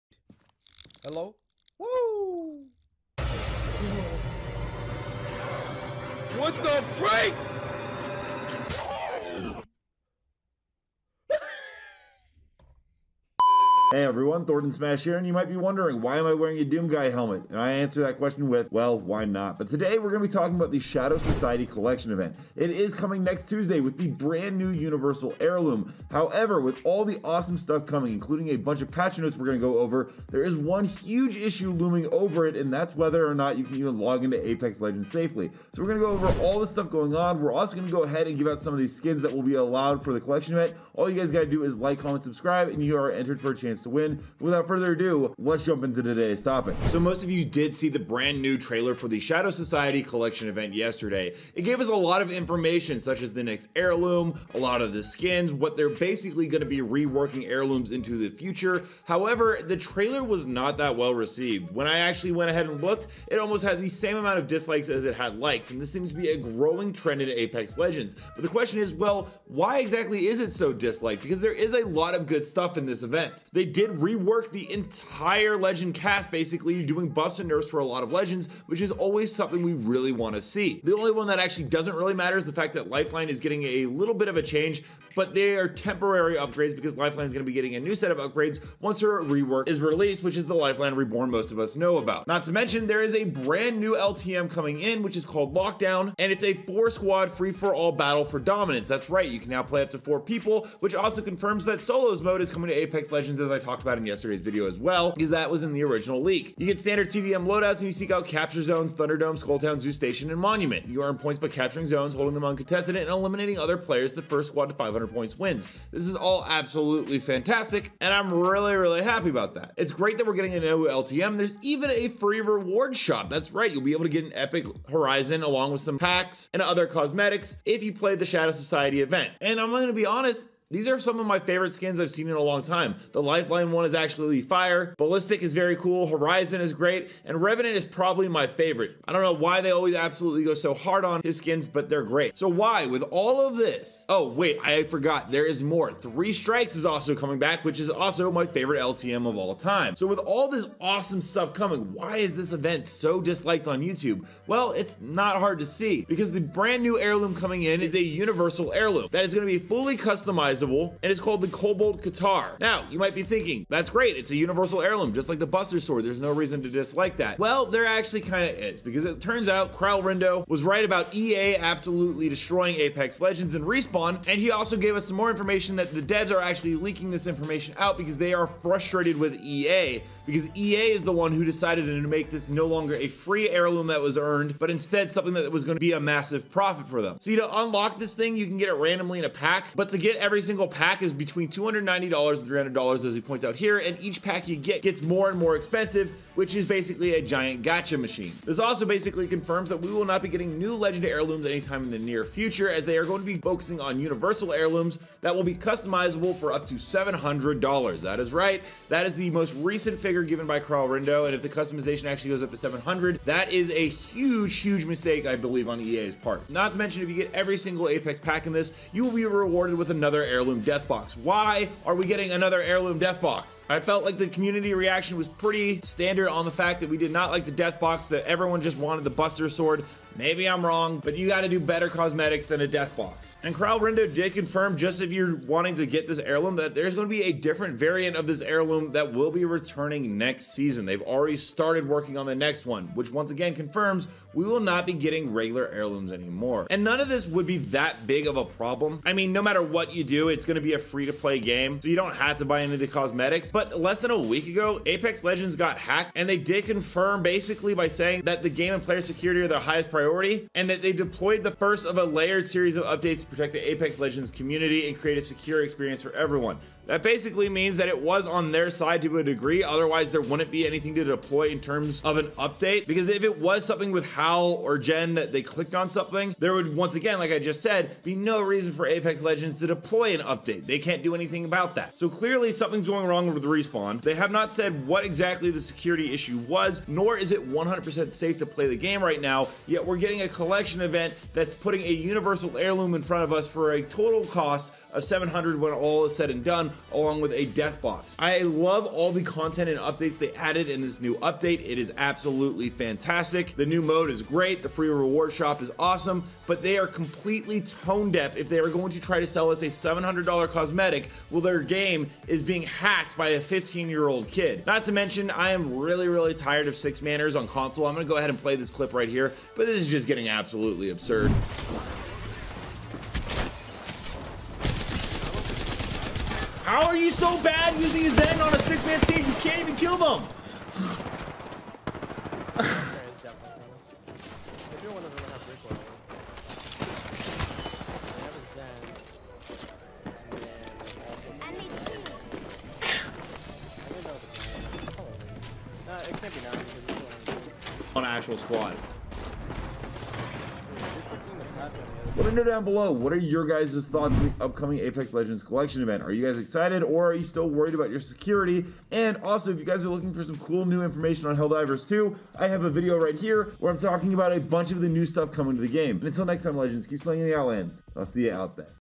Apex Legends New Collection Event Heirloom Has A Huge Problem
Apex Legends New Collection Event Heirloom Has A Huge Problem as we discusse the "Shadow Society Event" in Apex Legends and highlights several aspects that make it a potentially negative and greedy event. It mentions the high price of the event's cosmetics, which are significantly more expensive than previous offerings. The article also criticizes the limited-time nature of the event, creating a sense of urgency and potentially pressuring players to spend more money to obtain exclusive items before they disappear. Additionally, it mentions concerns about the event's gameplay changes, which may favor players who spend money on certain items, potentially creating an imbalance and a pay-to-win scenario. Overall, the article presents the "Shadow Society Event" as a potentially exploitative and profit-driven approach by Apex Legends developers, raising concerns among players about the game's direction and fairness to its player base.
**Giveaway winner announced at end of video, Direct Message on Twitter @thordansmash to claim prize if you are a winner!